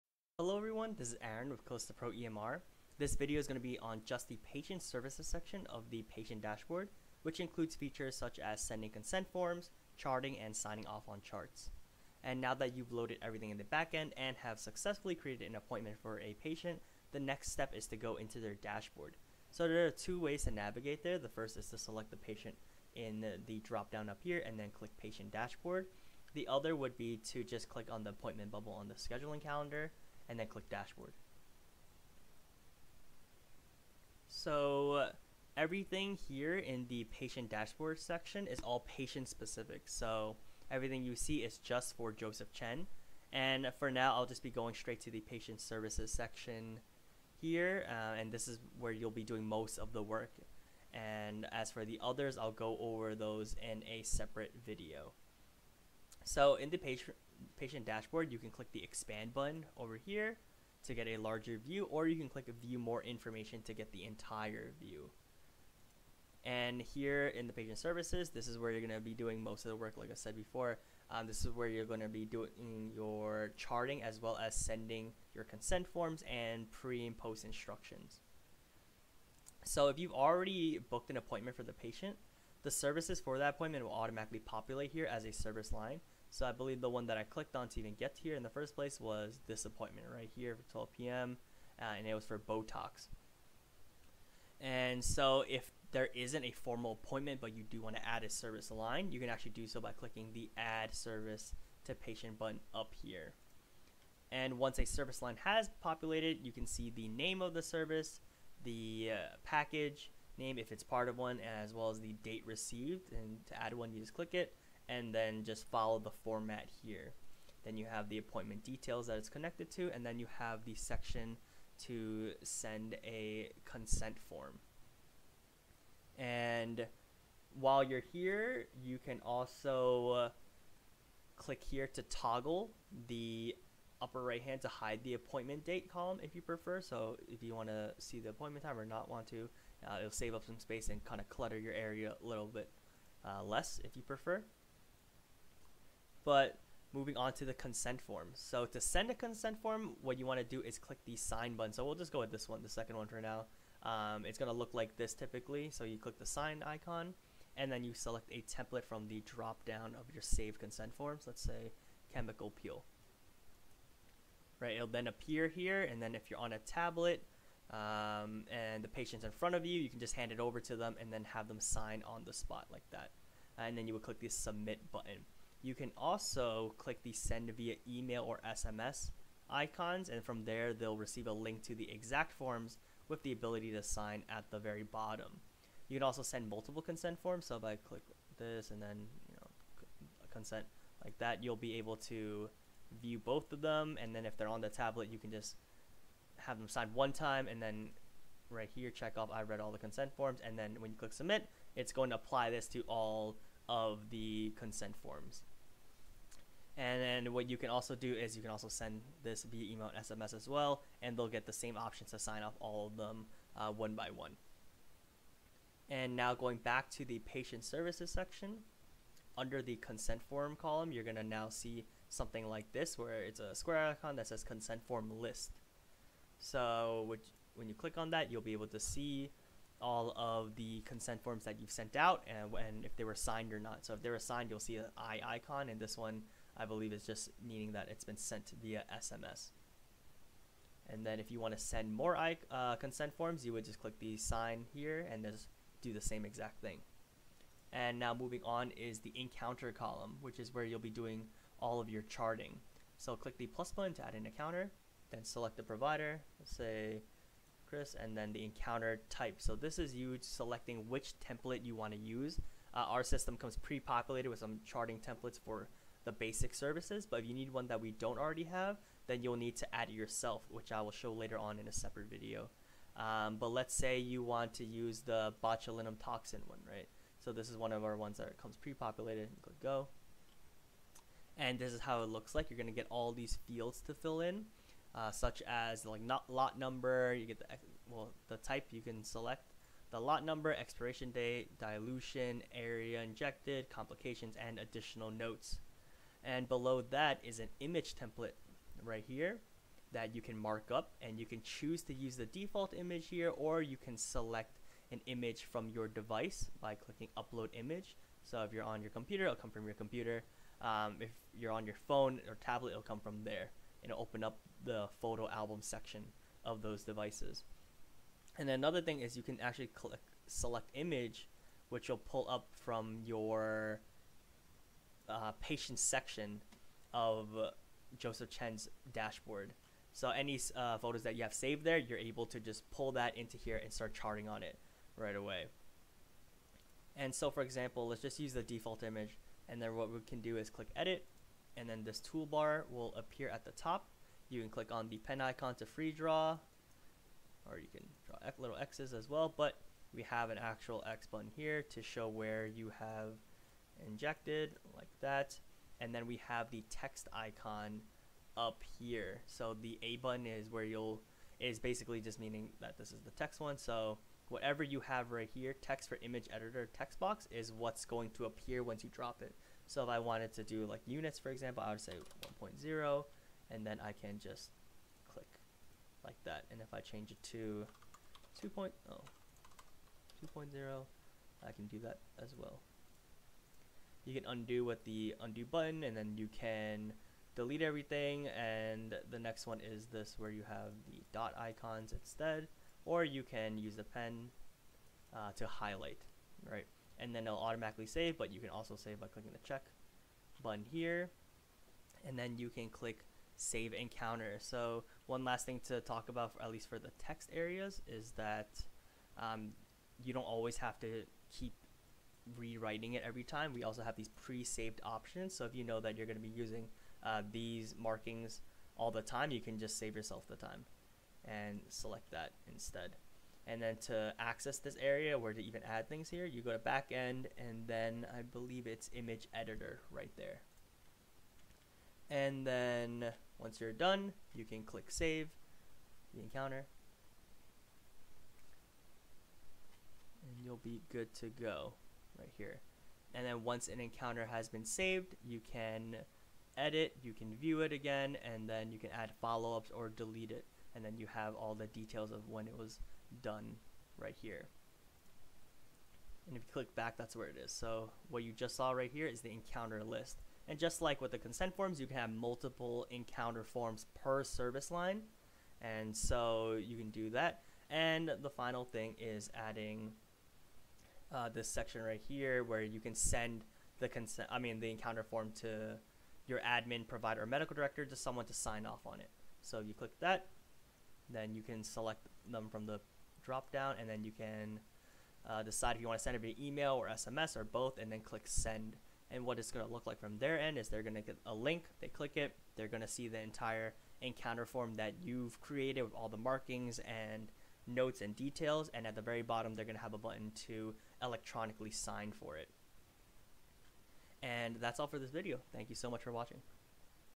8 Patient Dashboard Pt 1 Sending Consent Forms Charting and Co Signing
In this video I go over the Patient Services section of the Patient Dashboard. Includes how to send Consent Forms, Chart patient encounters, and Co-signing encounters

If you are interested in demoing or setting up an account please